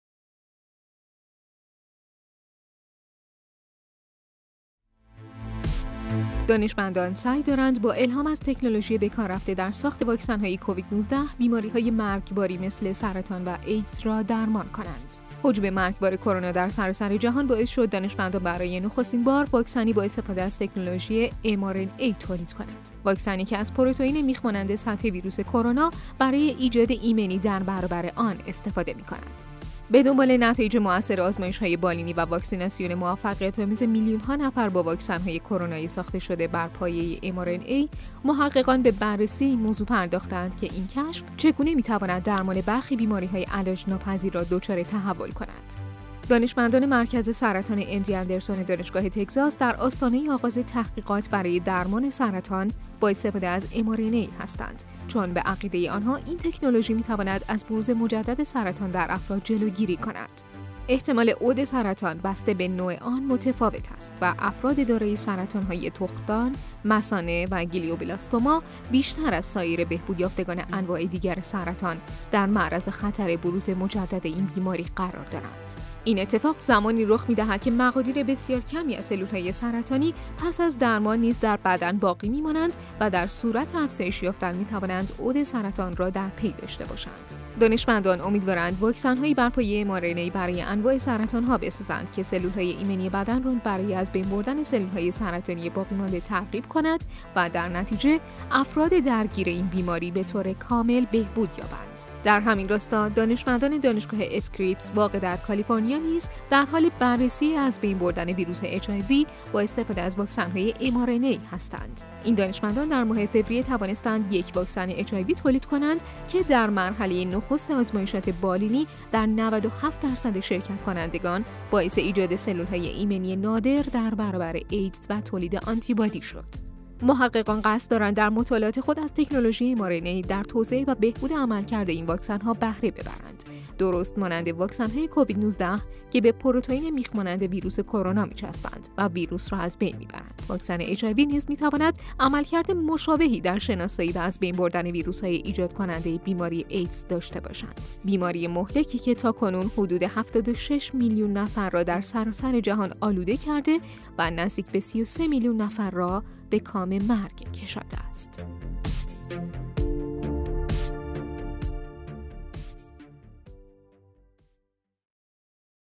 نبرد با ایدز و سرطان، با استفاده از تجربه کرونا
دانشمندان سعی دارند با الهام از تکنولوژیِ به کار رفته در ساخت واکسن‌های کووید-۱۹، بیماری‌های مرگباری مثل سرطان و ایدز را درمان کنند. هجوم مرگبار کرونا در سراسر جهان باعث شد دانشمندان برای نخستین‌بار واکسنی با استفاده از تکنولوژی mRNA تولید کنند؛ واکسنی که از پروتئین میخ‌مانندِ سطح ویروس کرونا، برای ایجاد ایمنی در برابر آن استفاده می‌کند. به دنبال نتایج مؤثر آزمایش‌های بالینی و واکسیناسیون موفقیت‌آمیز میلیون‌ها نفر با واکسن‌های کرونای ساخته‌شده بر پایۀ mRNA، محققان به بررسی این موضوع پرداخته‌اند که این کشف، چگونه می‌تواند درمان برخی بیماری‌های علاج‌ناپذیر را دچار تحول کند.